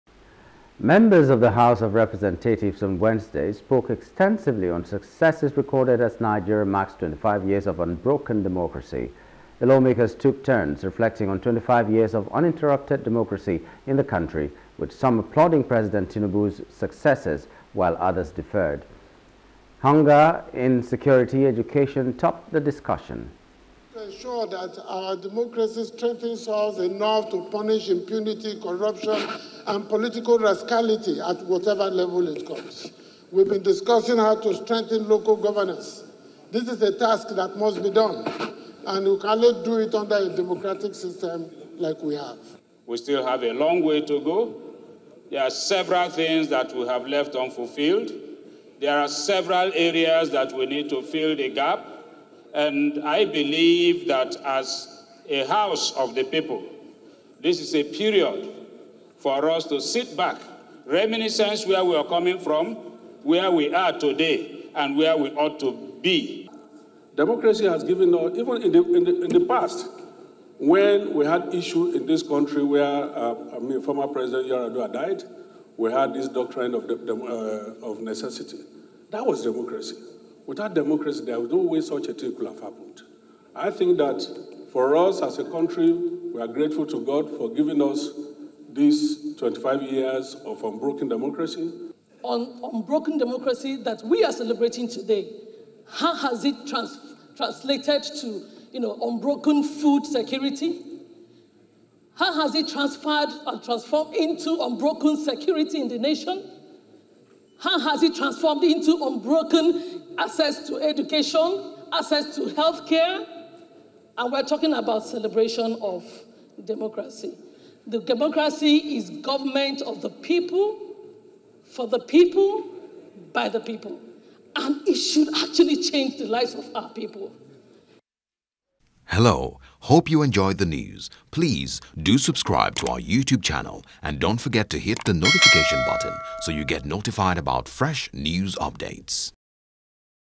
25 Years Of Unbroken Democracy: Lawmakers Differ On Success Recorded
Members of the House of Representatives on Wednesday spoke extensively on successes recorded as Nigeria marks 25 years of unbroken democracy.